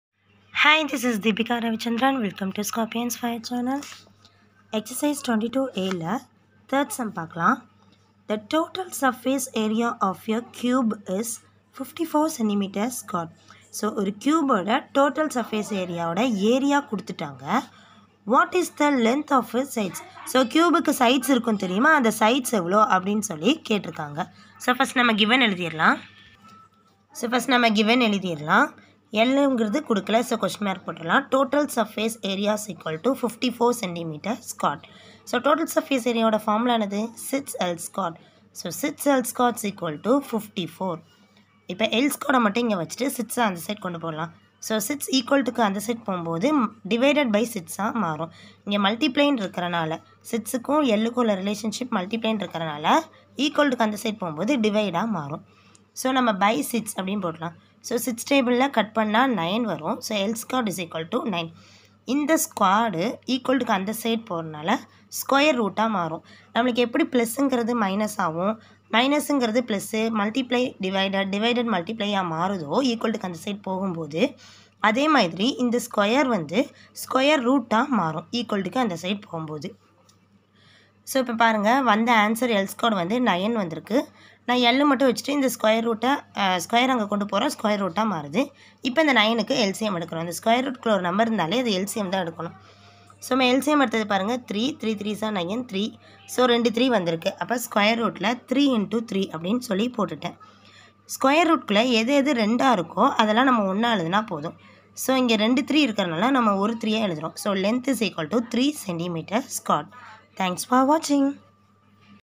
Ex 22(A) No.3
Maths - Std VIII - ICSE - NumbersWiz
Chapter 22(a) - volume and surface area of solids - Ex 22 (a) - No.3 in tamil